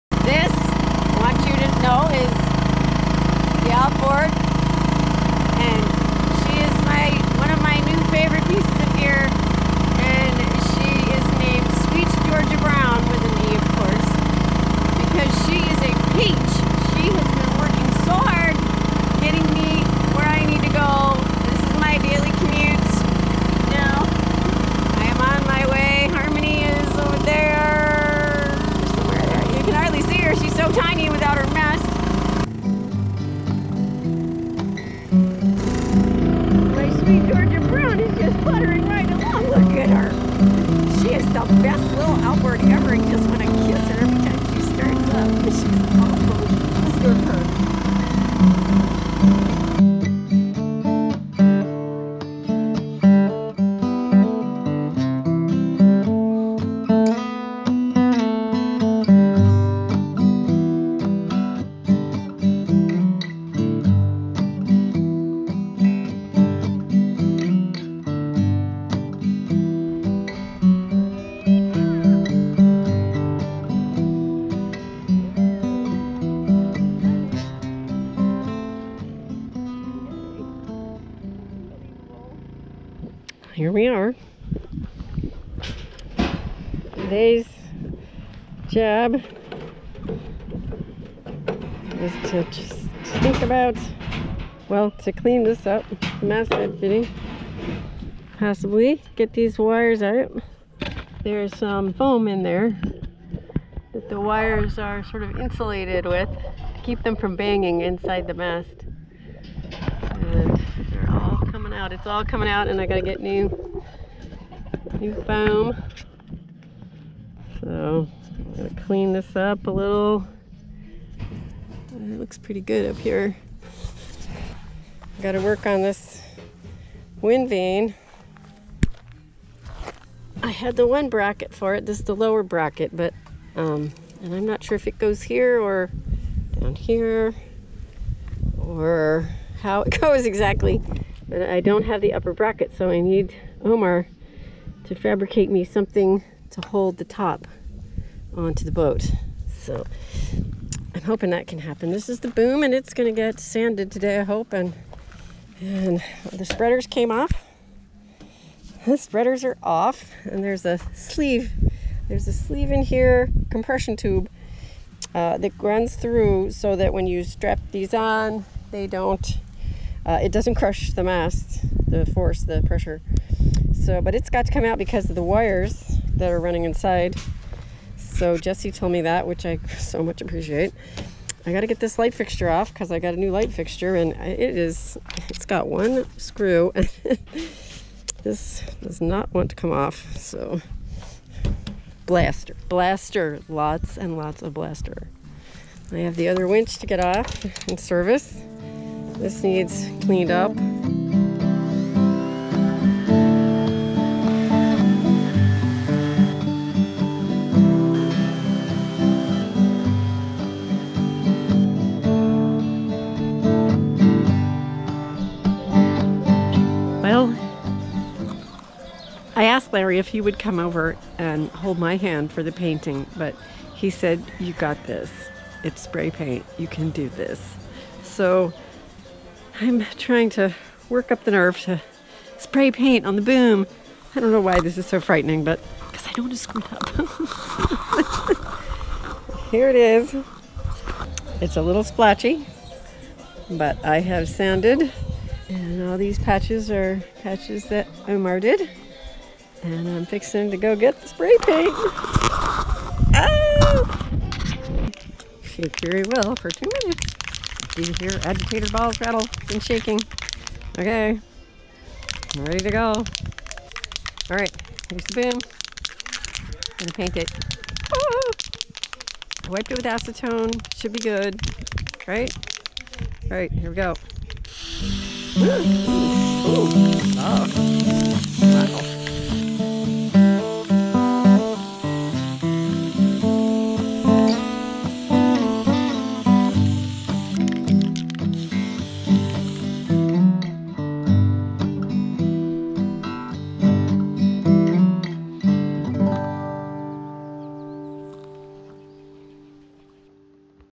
Commute to the Mast
This is a short one as I am getting back to editing after some amazing help the last couple of months.

In this episode, I commute to the mast and get to work on the boom.